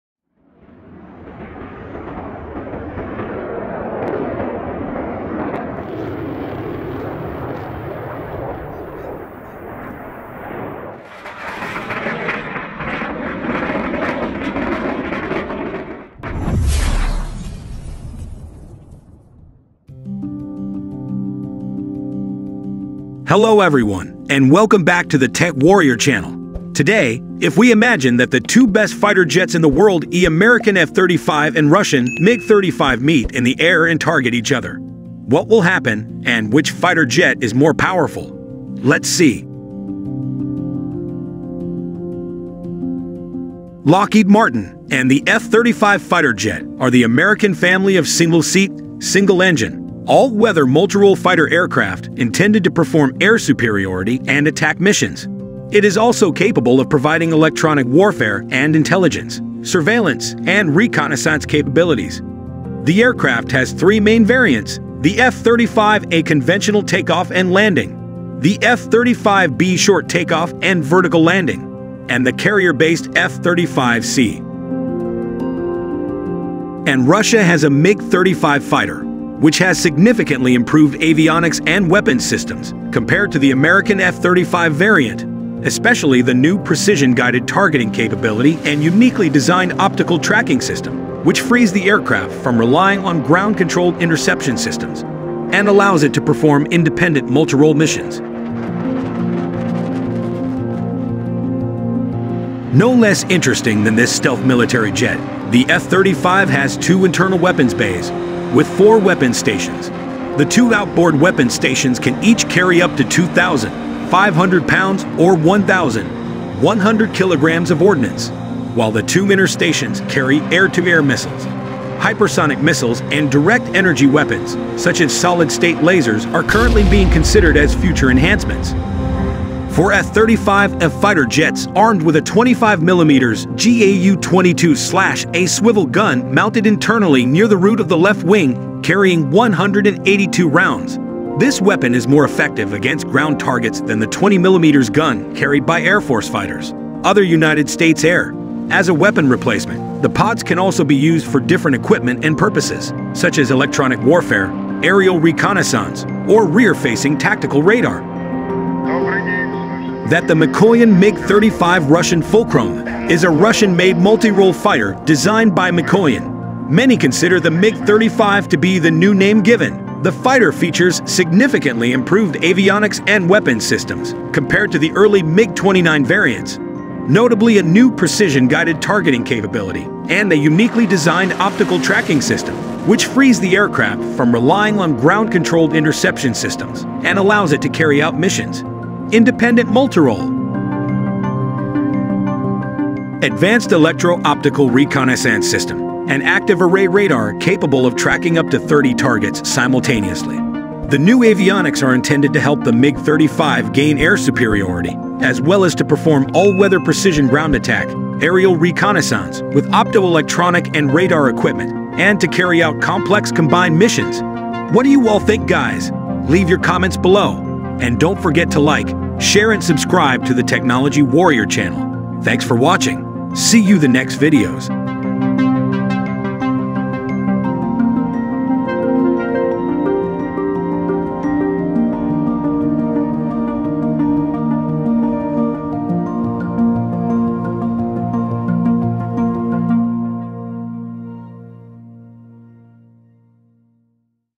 These Russia New Fighter Jet Can Beat The American F-35 Stealth Fighter
Armament The MiG-35 is likely to use laser armament in the future. Advanced electro-optical reconnaissance system, and active array radar capable of tracking up to 30 targets simultaneously. The MiG-35 is also capable of launching Kh-36 Grom-1 cruise missiles, giving the enemy a long-range strike capability. Russia has a MiG-35 fighter which has significantly improved avionics and weapons systems compared to the American F-35 variant.